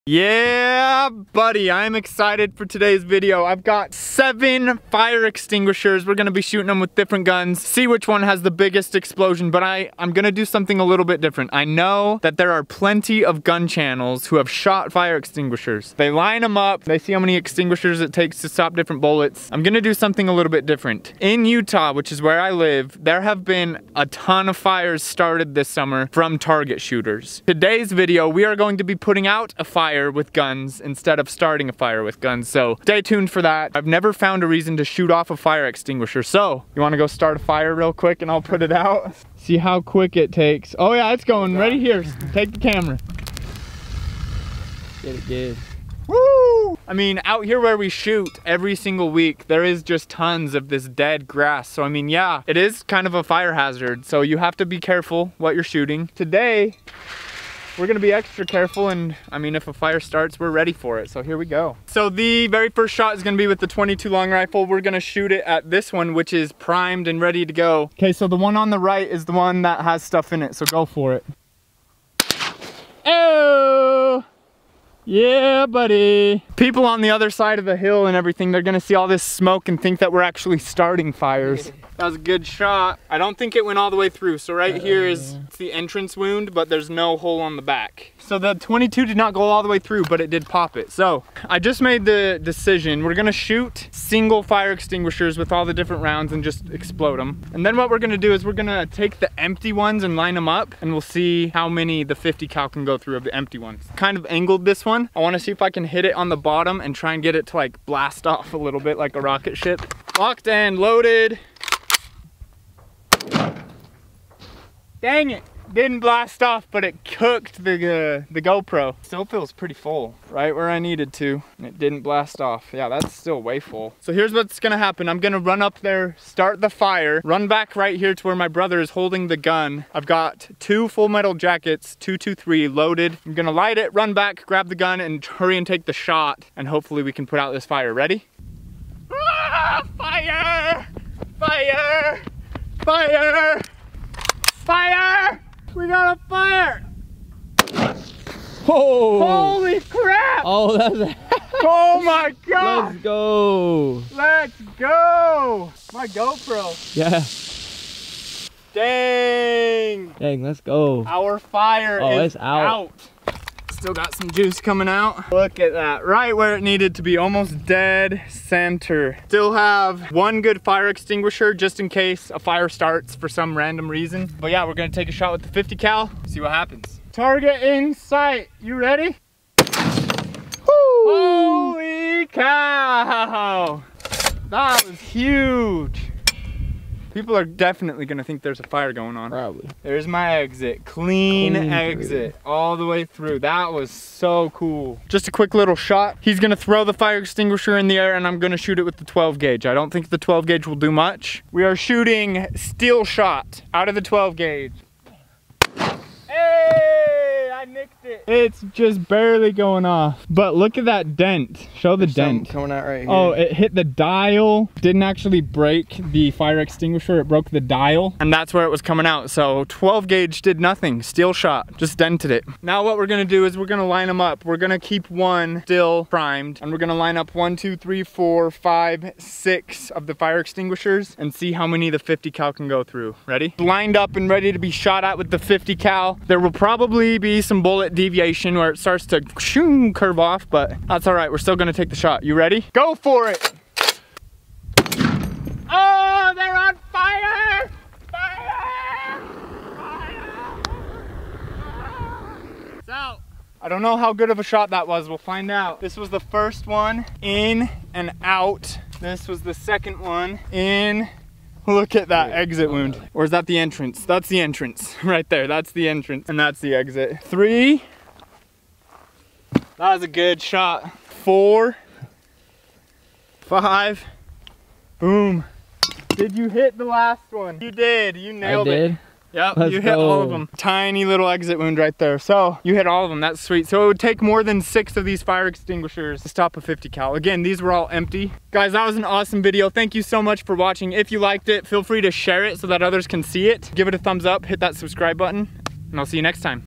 Guns VS Fire Extinguisher! Fire Extinguisher Goes BOOM!
50 cal vs fire extinguisher! I had a bunch of fire extinguishers that I decided to shoot with different guns! I even started a fire to see if you can shoot a fire extinguisher and put the fire out. Let me know what else you want to see me shoot!